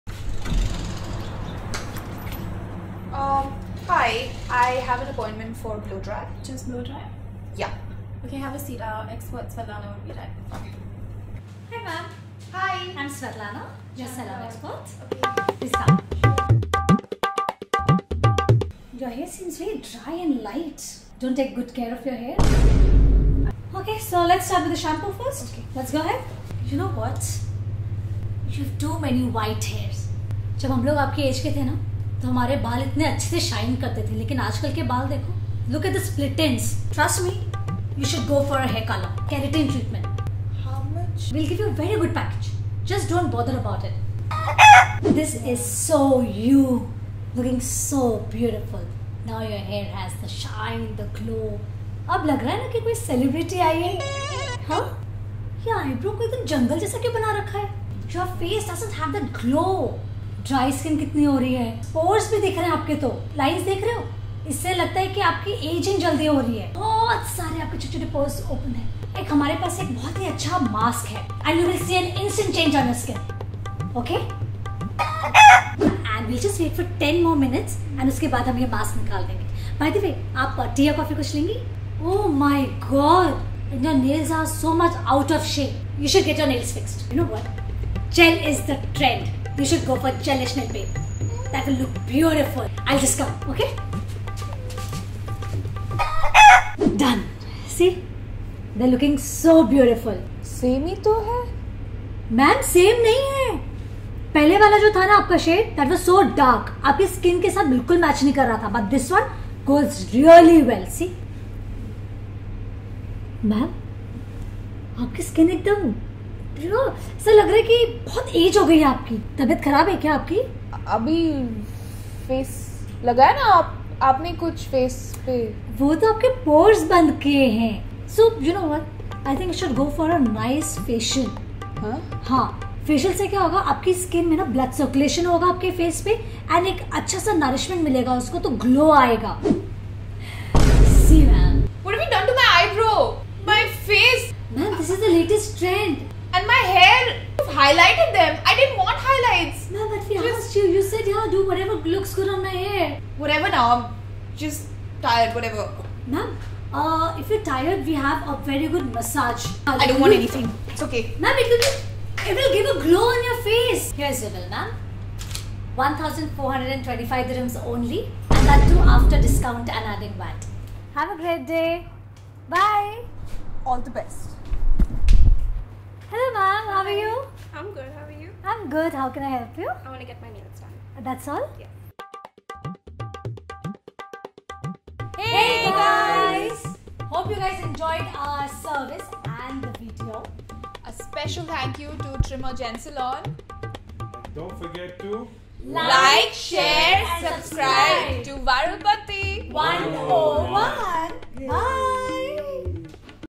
Every Beauty Parlour Waali is a Sales Agent! I Viralpanti 101
WATCH What happens when you walk into a Ladies Beauty Parlour :P
Every Ladies Parlour Waali is a Sales Agent trying to sell you their products and services.

FT: Zhankar Gandhani & Svetlanna K Meshra
Location Courtesy: trimer gents salon
Hair & Make Up: Rachel Suzanne
Camera & Edit: Zubair Ahsan Barkaat
Directed by & Isac Thomas
Produced by Viralpanti101